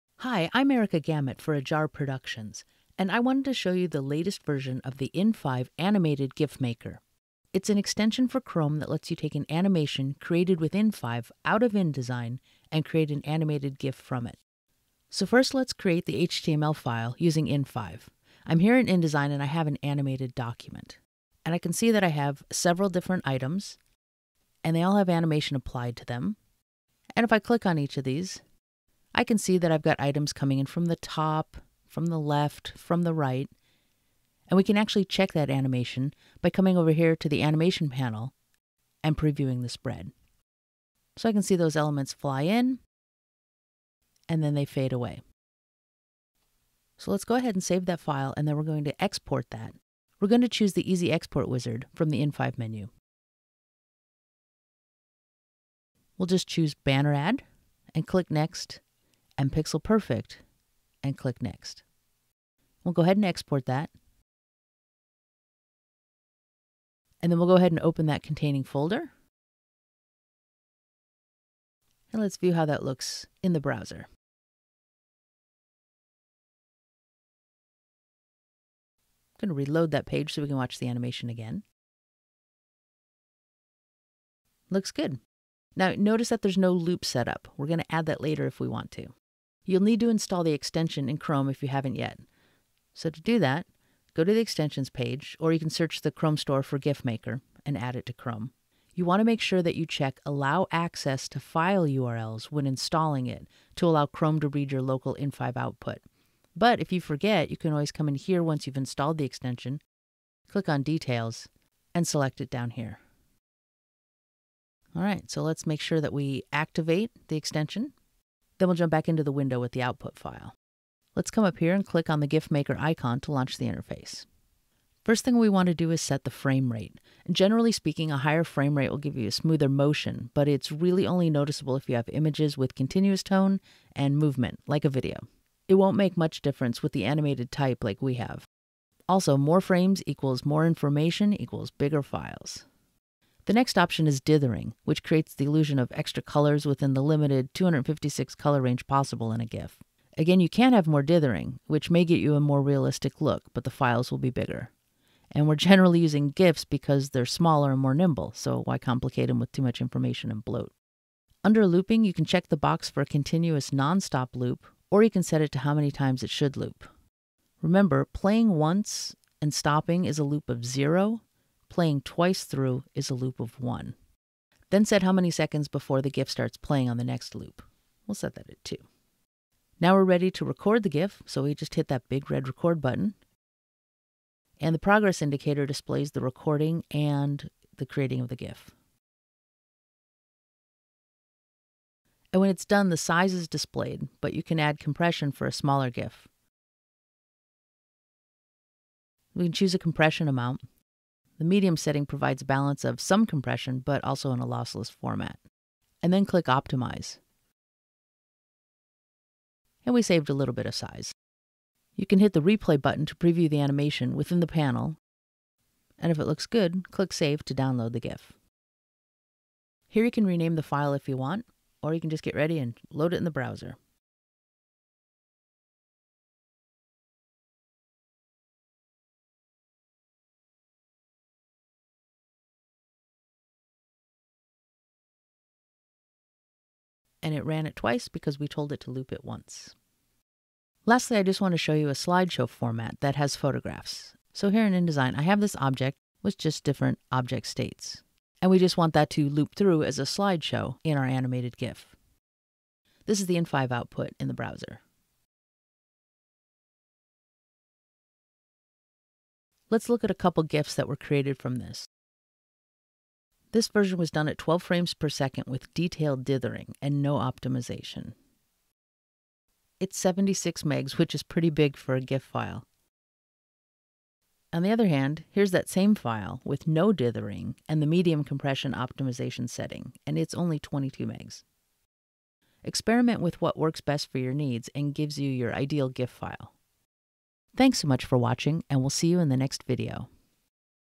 Using the in5 Animated GIF Maker Extension v2
The Animated GIF Maker is a Google Chrome extension that lets you create animated GIFs from InDesign-exported content.